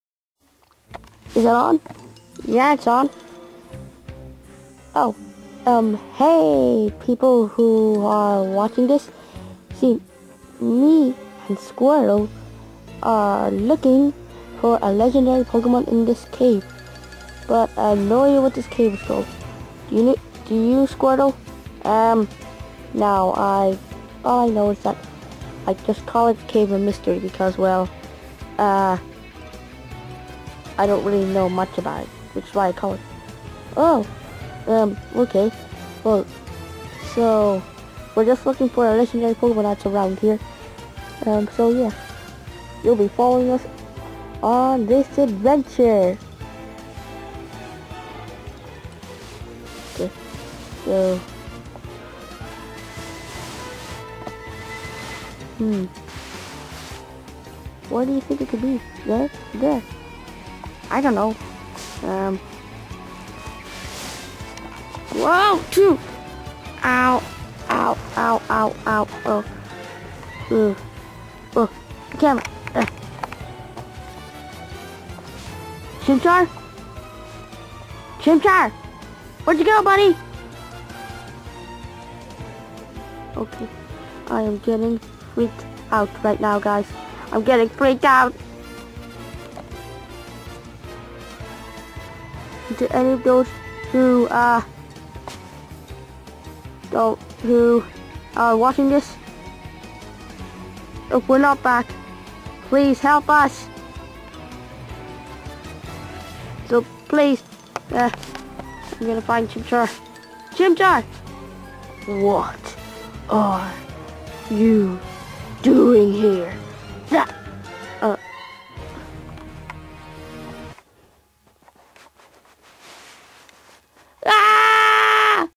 TPA (ser1 ep9) sneak peak trailer
This is a trailer or wahtever you want to call it, on episode 9 so this will be a speacial video because this will be an 1 year anniversary video! It will be posted up on the 1st of July. Any way hope you enjoy the video.